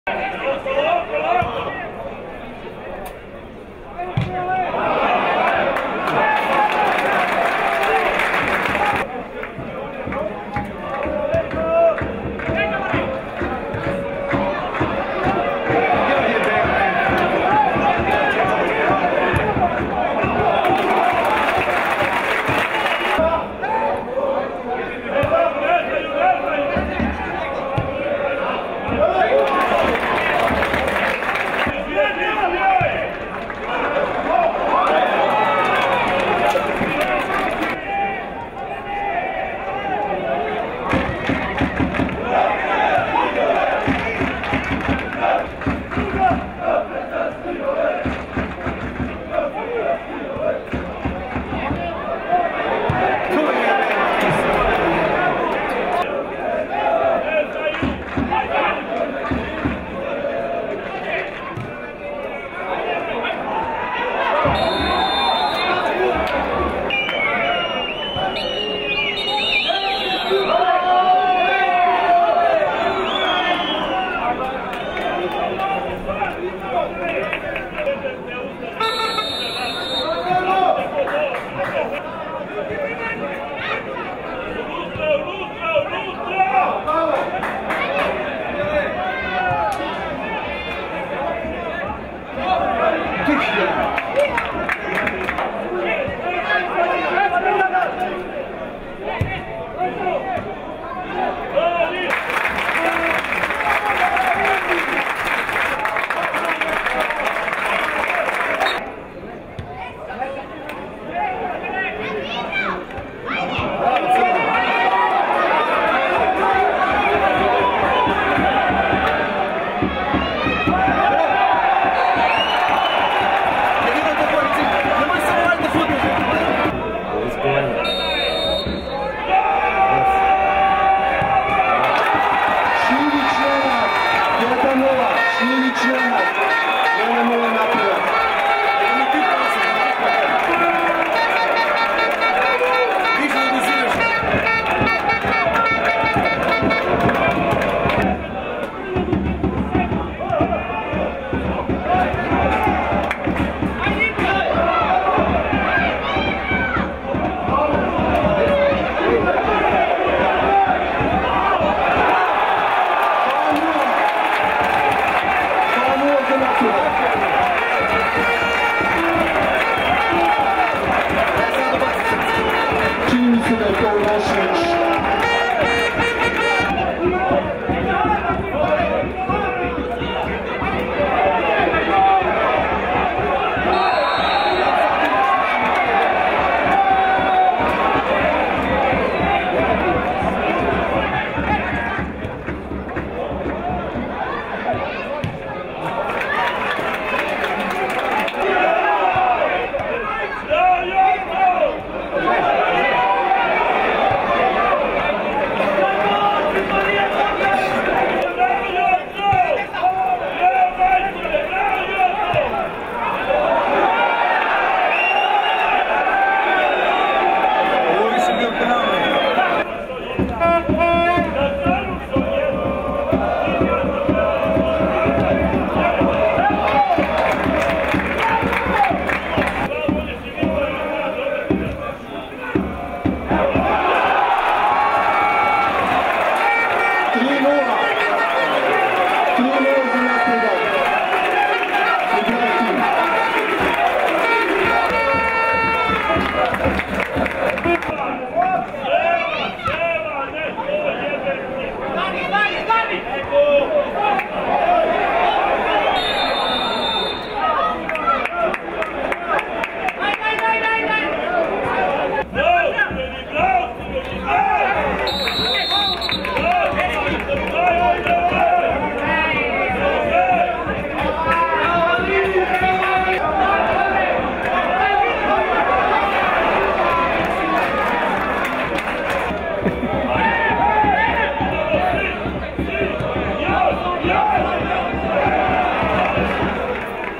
Superliga 2016/17, 5. kolo plej - ofa: Napredak - Javor 3:0
Strelci: 1:0 Šljivić u 60 (P), 2:0 Urošević u 66, 3:0 Ndiaye u 81. minutu